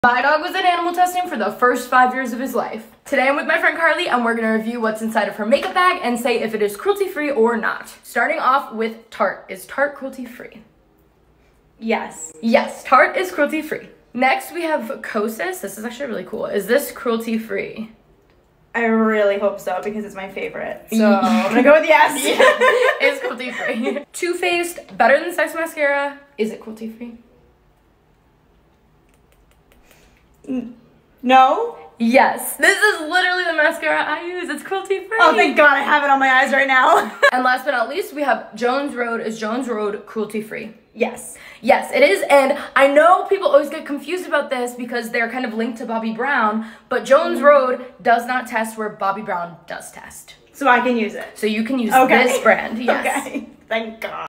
What’s inside my friends makeup bag that she should LOSE?
What should my bestie Carly use and lose? If it’s not cruelty free we use a process called use it and lose it. This replaces non cruelty free products with cruelty free dupes as they become empty! Buying new makeup is fun and can be affordable as long as you do it slowly and shop brands in your budget!